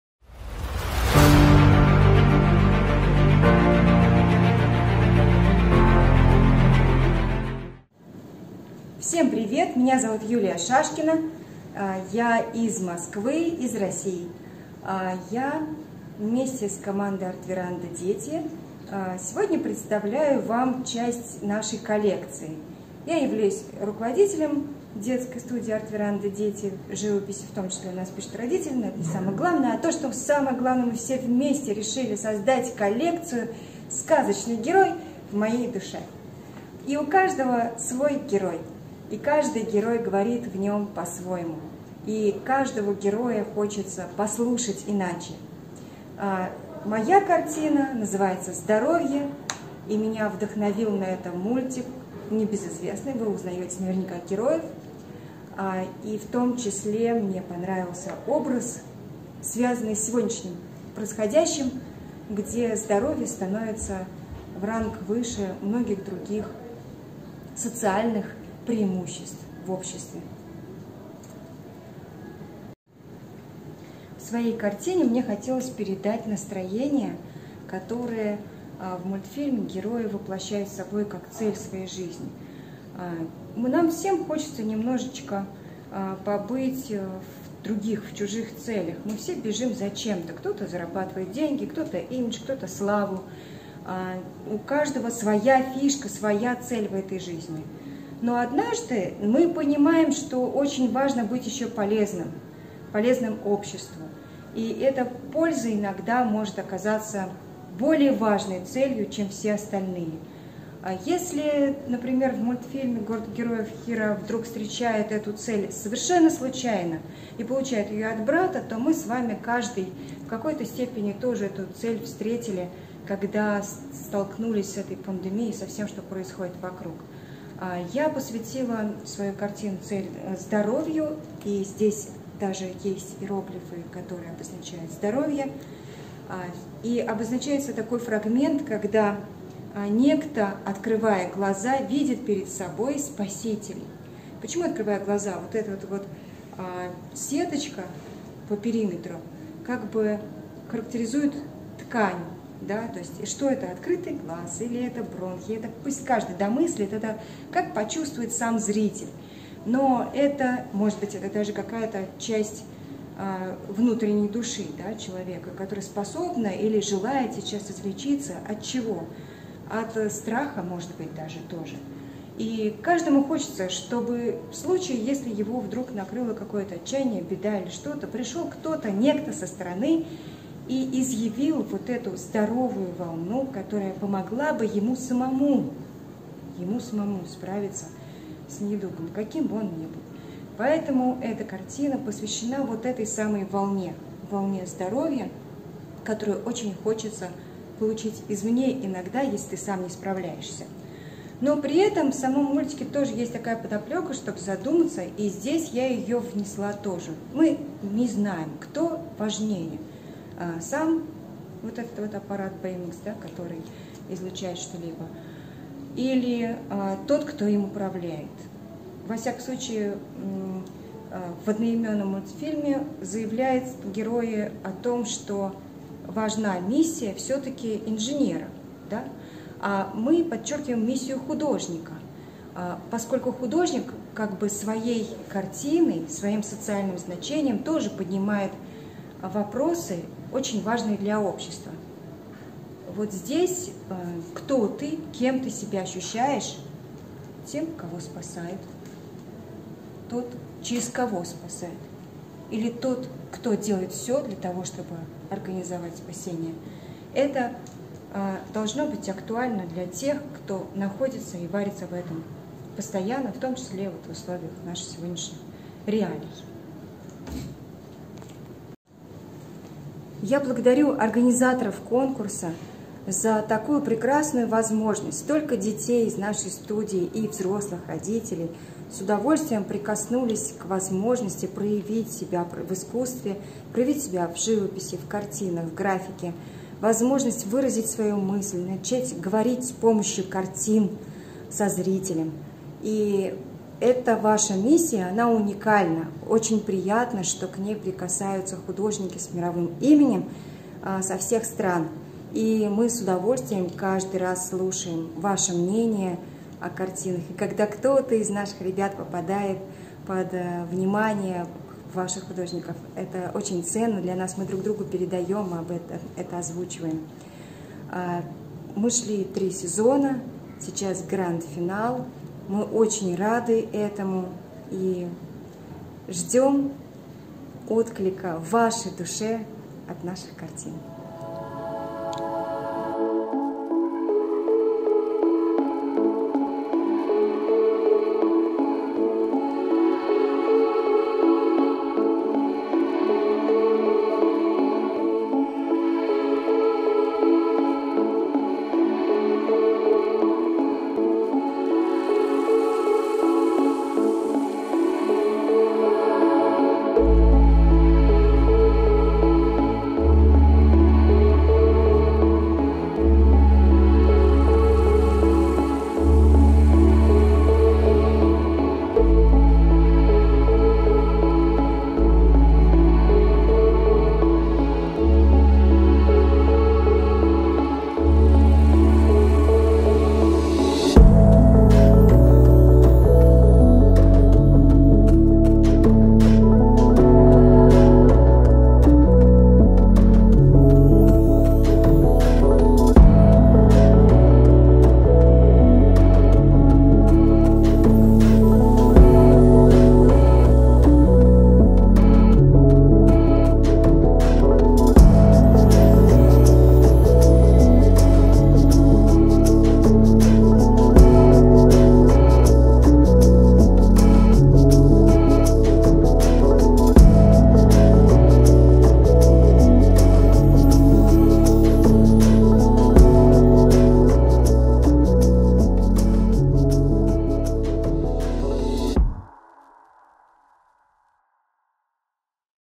Golden Time Talent | GRAND FINAL | Julia Shashkina Art veranda deti | GTGR-2788-8657
Имя участника или коллектив: Julia Shashkina    Art veranda deti

Страна: Russia

Город: Москва

Название конкурсной работы: Сказочный герой в моей душе. Здоровье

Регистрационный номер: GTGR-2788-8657

Номинация: Живопись (взрослые) от 18 лет

GRAND FINAL

Участие в других конкурсах и фестивалях / творческие достижения: Юлия Шашкина. Картины давно уже пишу под заказ и основной фонд моих работ находится на руках заказчиков. А в студии Арт-веранда.дети под моим руководством создаются картины, которые уже обладают авторством исплонителей-авторов. Дети предложили мне тоже подключиться к международному конкурсу и я решила поддержать их инициативу. Ведь если учитель не боится конкурсов, то и детям есть куда стремиться. Мы участвовали три сезона с хорошими результатами всей командой. Получив приглашение к Grand Final, нам пришла идея создать коллекцию, которая, как мне кажется, еще покажет  себя миру в полном составе. Мы хотели поговорить со зрителем своей коллекцией о заветных качествах души и приглашаем вас к этому диалогу.